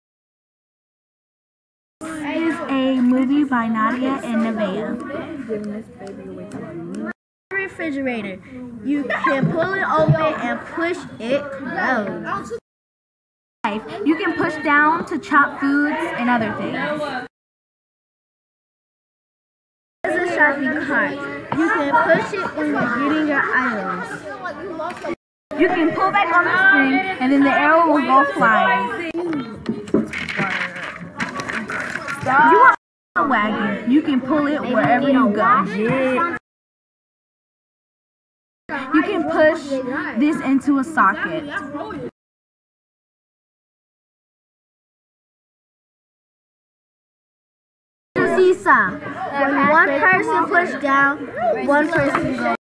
my movie by nadia and kelis and neaveh
Blackwell's 4th grade class
Force of Motion project